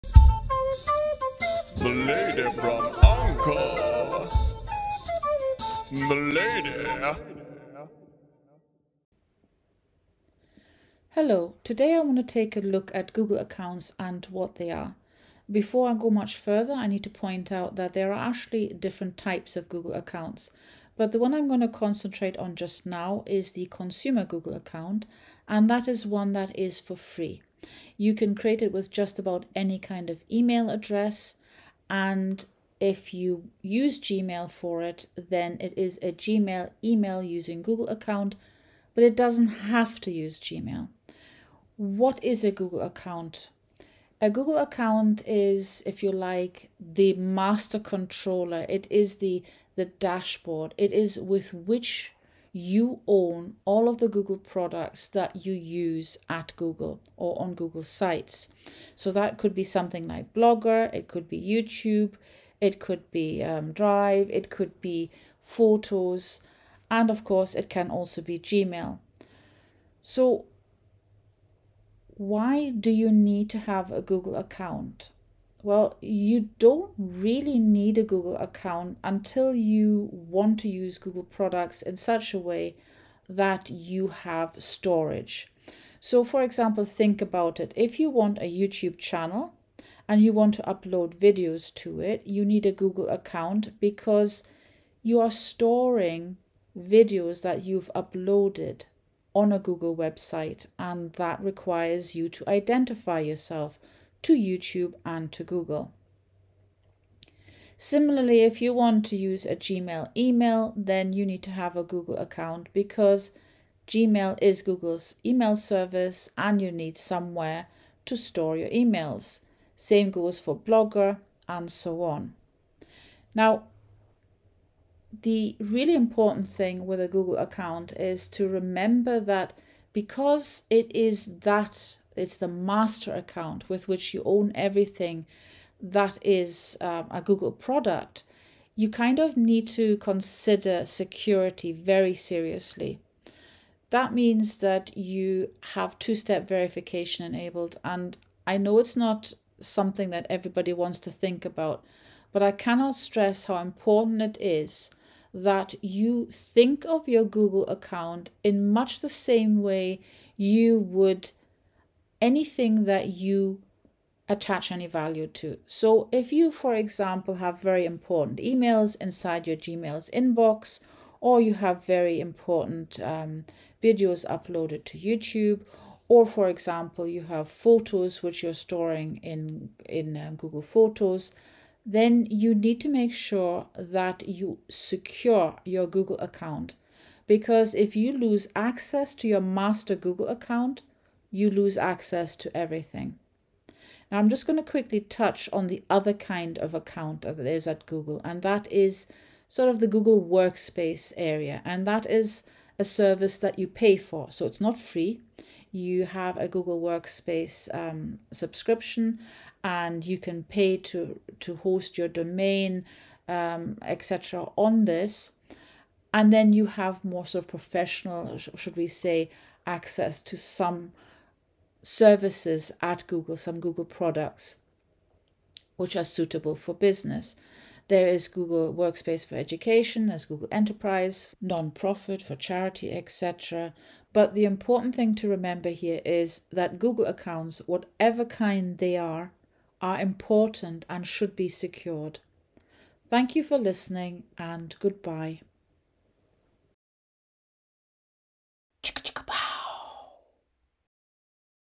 What Exactly Is A Google Account?
A Google Account is a master account, a"controller" for Google products such as YouTube, Gmail email, Google Photos and so on.

The information-rich videos are meant to help you in finding your own solutions.  This because if I just say "do this, do that", it wont help you avoid these issues in the future.  As they say-  "knowledge is power".

You can usually find me on the Google help forums